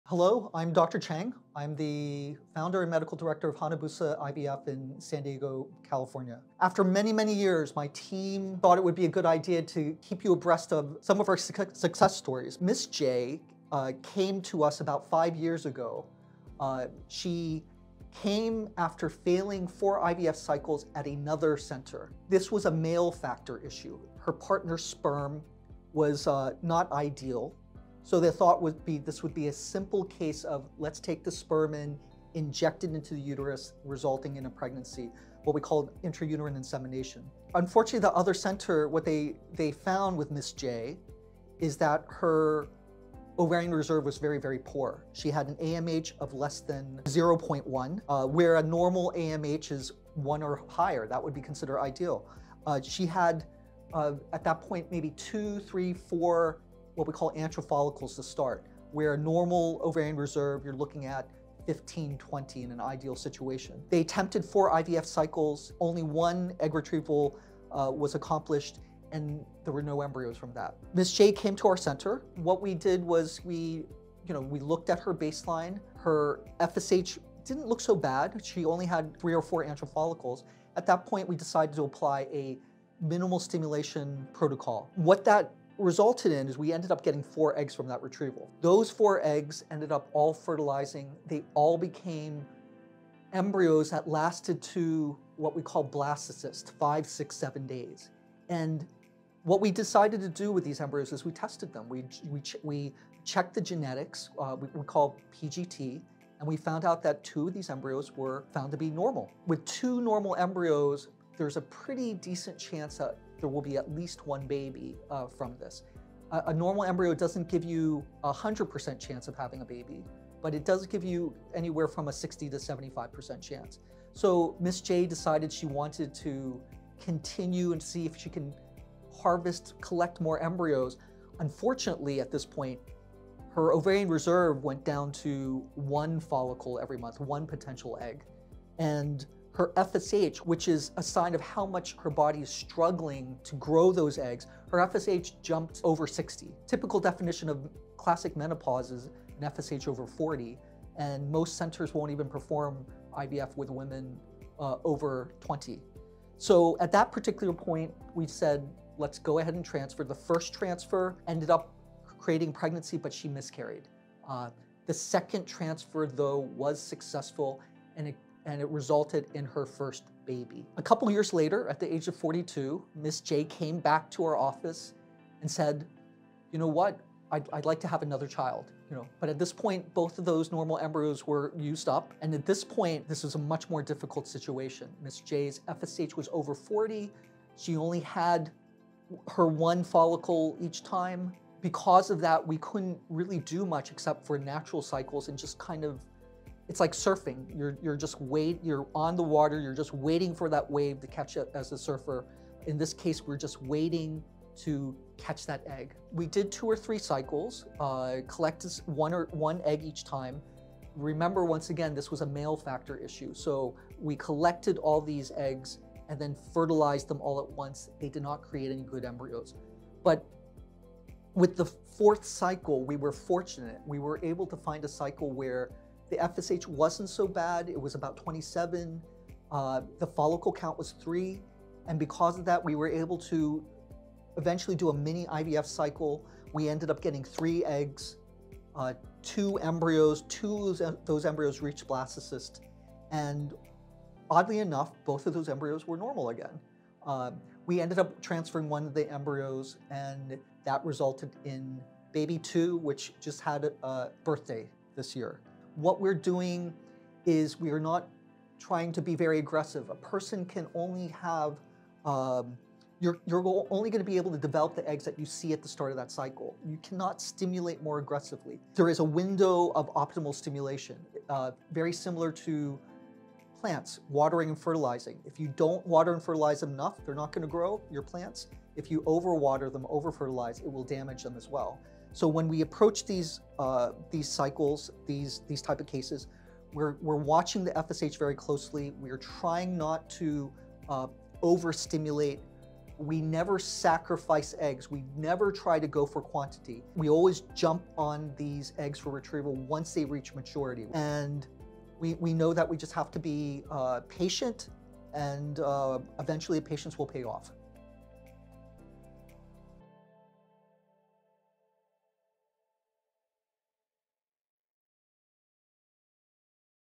Patient Success Story - How Minimal Stimulation IVF Can Help with a Poor Ovarian Reserve
In this inspiring video, Dr. Lyndon Chang shares another incredible success story from Hanabusa IVF in San Diego

Meet Ms. J, a patient with a remarkable fertility journey despite having the odds stacked against her.

Ms. J's challenges were two-fold with a male factor issue coupled with a very poor ovarian reserve which resulted in multiple failed IVF cycles at other centers. However, Dr Chang’s team took a unique approach by implementing a minimal stimulation protocol that resulted four healthy embryos.

Through preimplantation genetic testing, it was found that two of the embryos were normal. Although the first transfer was unsuccessful, the second transfer led to a successful pregnancy and the birth of her first child. However, her journey did not end.

At the age of 42, Ms. J returned to try for a second child. Ms. J had high FSH levels, and one follicle each month. Through patience and determination, Ms. J and Dr. Chang worked together through her natural cycles to collect one egg at a time.

After three cycles, the fourth cycle proved to be fortunate where Ms. J’s FSH levels were lower and her follicle count was higher. Dr. Chang retrieved three eggs, two of which developed into normal embryos. After a successful embryo transfer, Ms. J welcomed her second child who just had their first birthday.

Beating all odds, Ms. J welcomed two beautiful children with the help of Hanabusa IVF.

Here’s what you’ll learn about Ms. J’s treatment:

0:00 - Introduction and background of Ms. J’s case

1:15 - Explanation of the treatment process - Minimal stimulation protocol

2:36 - The First Success

3:02 - Challenges for a Second Child - Natural Cycles

4:15 - Conclusion and lessons learned

Join Dr. Chang in this video as he shares valuable insights on the importance of exploring all options and personalized approaches to fertility treatment.

Discover how a low complexity IVF cycle can offer a less invasive, cost-effective, and empowering solution for individuals facing similar challenges.

The Hanabusa IVF team is committed to supporting you and doing everything possible to make your dream of building a family come true.